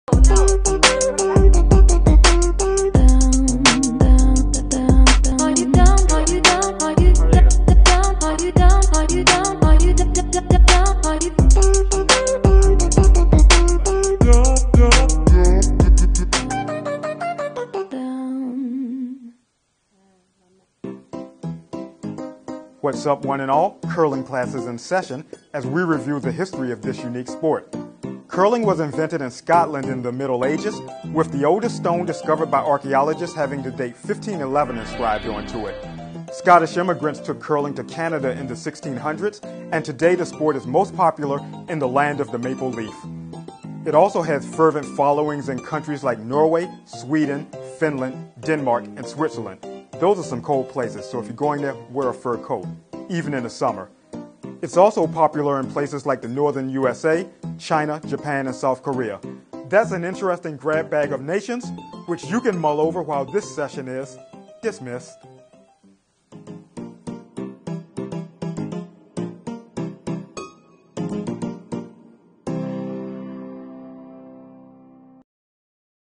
English Curling Class 1: Do you know the history of curling？
Sports Scene commentator Damion Jones teaches you English about curling: Do you know the history of curling? Where did it come from?